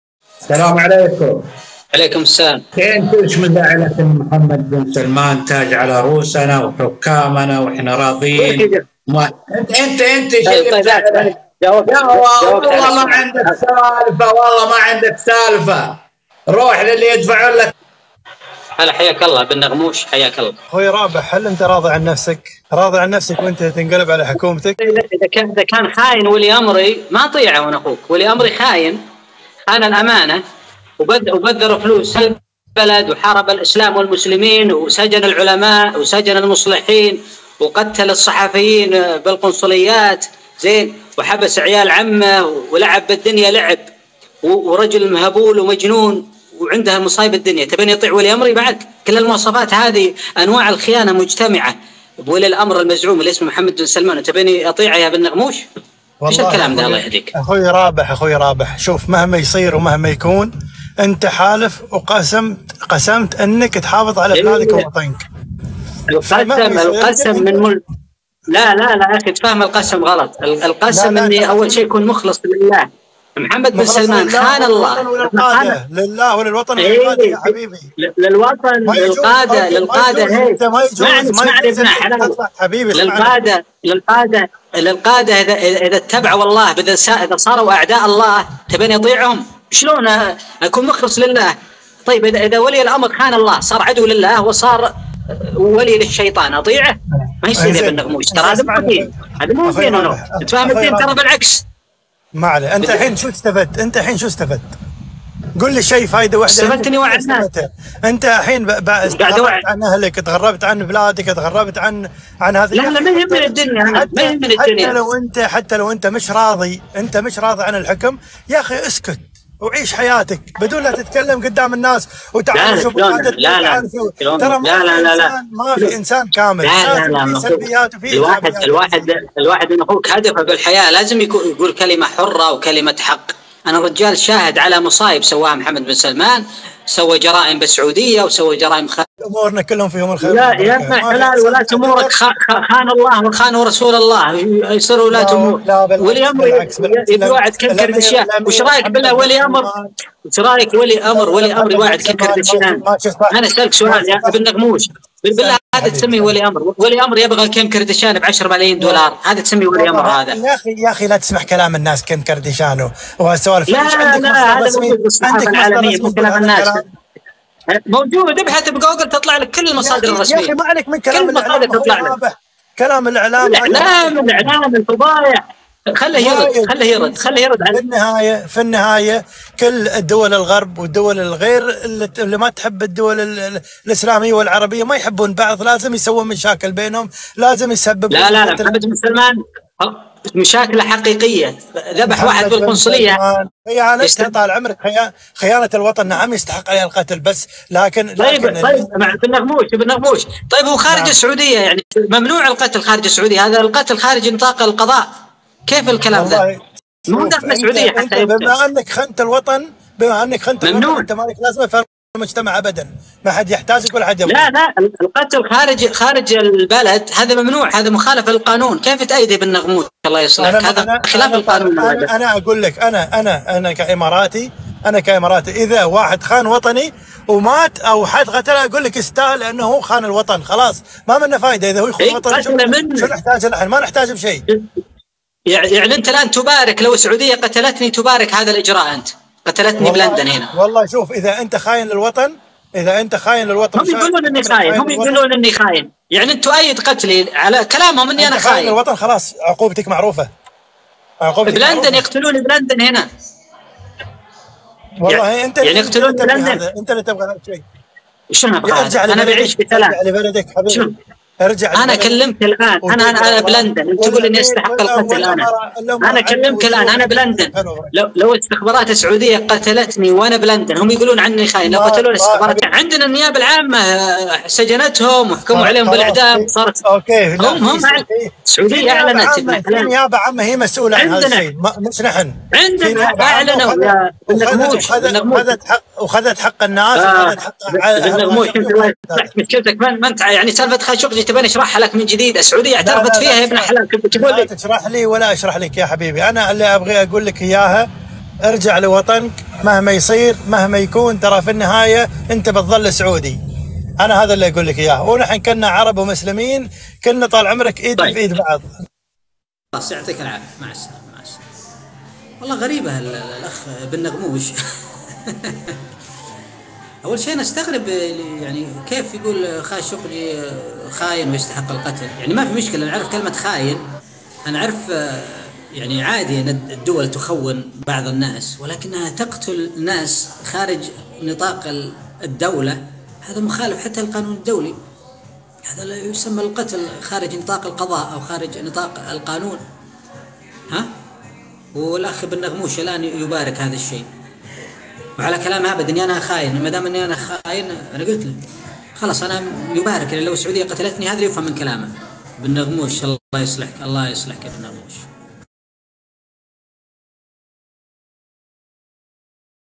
رابح العنزي 🔥 قست ناري مع 🔥بن نغموش🔥 اماراتي
فيديو جديد لـ العقيد السعودي المنشق رابح العنزي
ملخص بث رابح العنزي يوم 5-5-2023